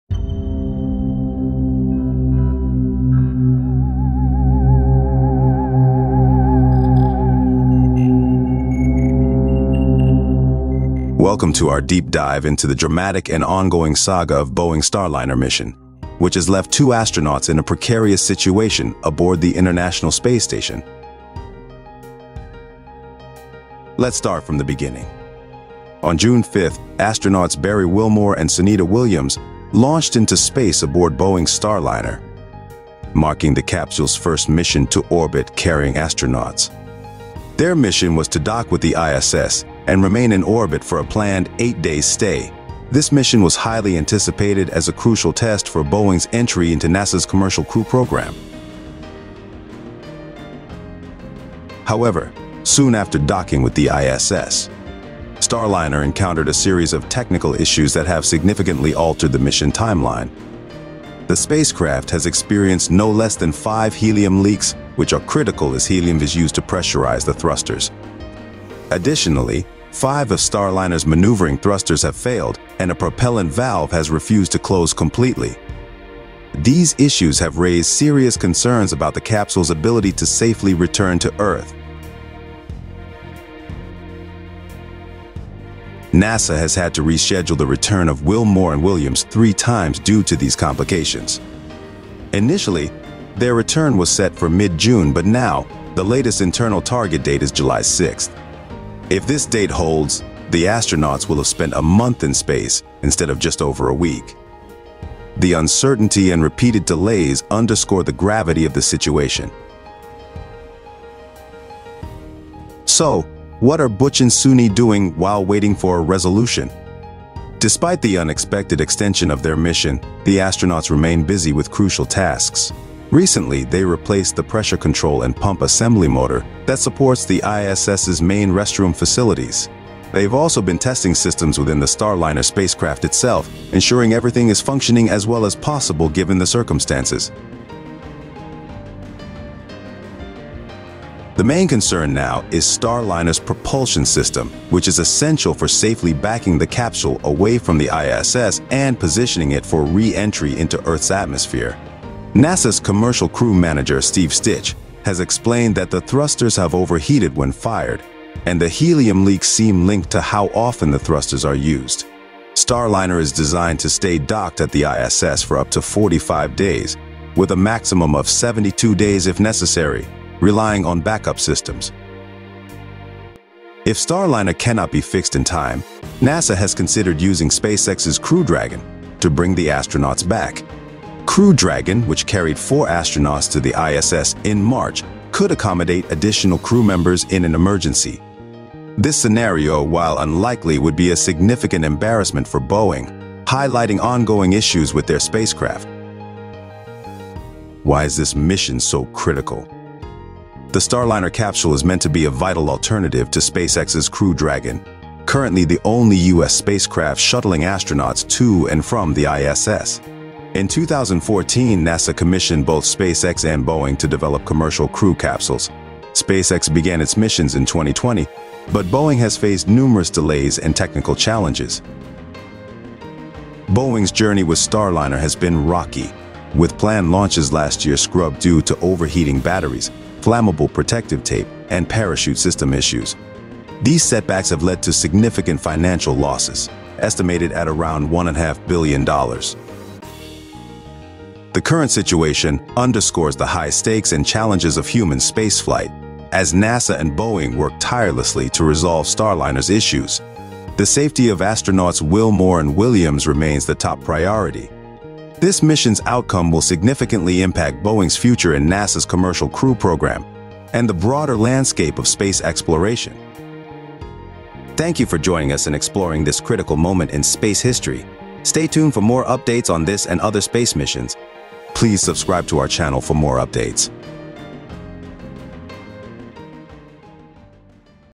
Stuck in Space: The Boeing starliner Crisis Explained! spacex |SpaceNews EP #22
What went wrong with NASA's Boeing Starliner Mission?
Two astronauts, Barry "Butch" Wilmore and Sunita "Suni" Williams, are currently stuck in space because Boeing's Starliner capsule is unable to make the return journey to Earth. They launched on June 5th in Starliner's first mission carrying astronauts. After docking with the International Space Station (ISS), they were scheduled to stay for eight days. However, the capsule has encountered multiple issues, including five helium leaks, five dead maneuvering thrusters, and a propellant valve that failed to close properly. NASA has rescheduled their return three times, with the latest tentative date being July 6th. These problems are primarily related to Starliner's propulsion system, which is necessary for detaching from the ISS and re-entering Earth's atmosphere. Despite these challenges, the astronauts have been busy performing maintenance tasks and system tests. If Starliner cannot safely return them, they may use SpaceX's Crew Dragon as an emergency backup. This mission is vital as Starliner provides an alternative to SpaceX's Crew Dragon for transporting astronauts. Boeing has faced significant setbacks, contributing to estimated losses of around $1.5 billion on the Starliner program.